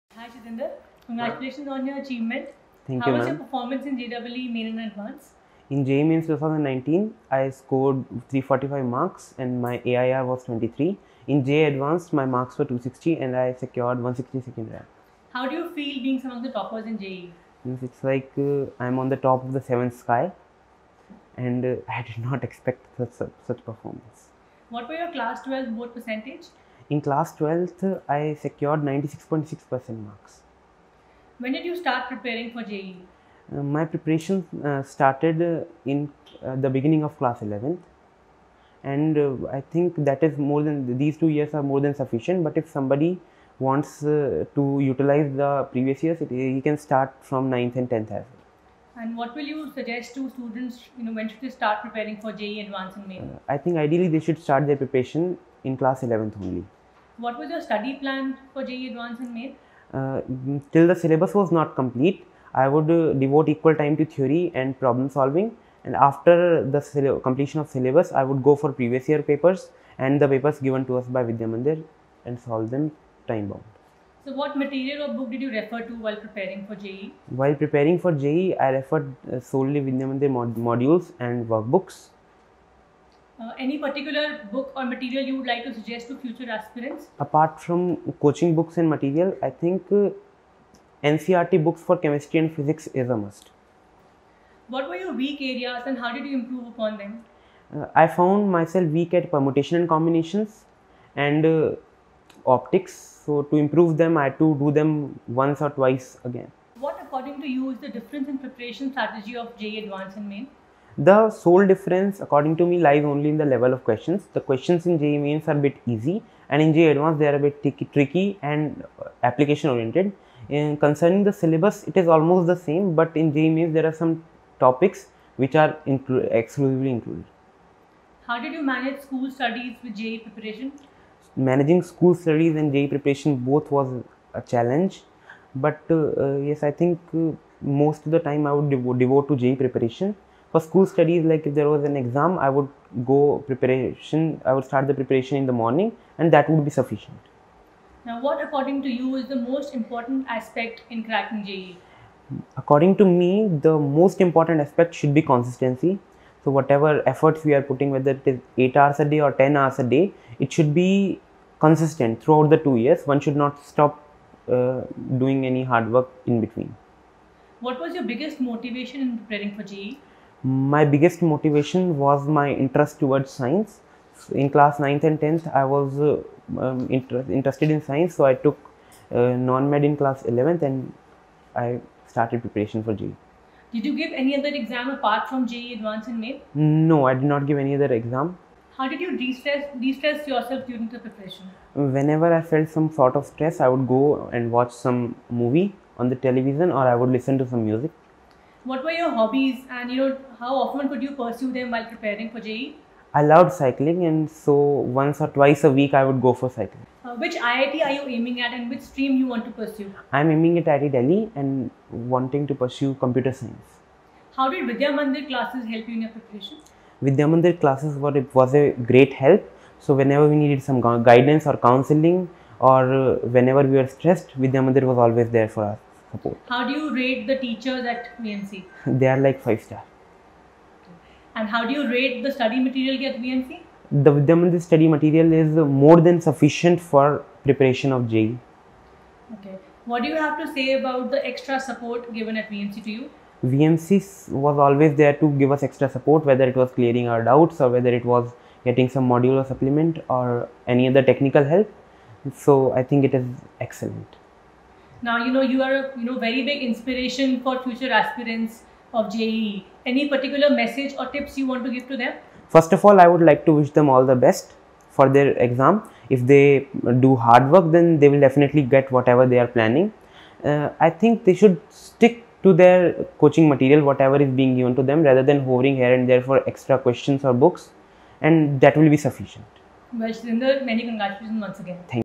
Success story of Jitender Kumar AIR 162 in JEE Advanced 2019
Jitender Kumar, a student of Two year classroom program in Vidyamandir Classes got a superb Rank of AIR 162 in JEE Advanced 2019 and AIR 23 in JEE Main 2019. He shares Tips and tricks to get a good Rank in JEE Advanced and JEE Main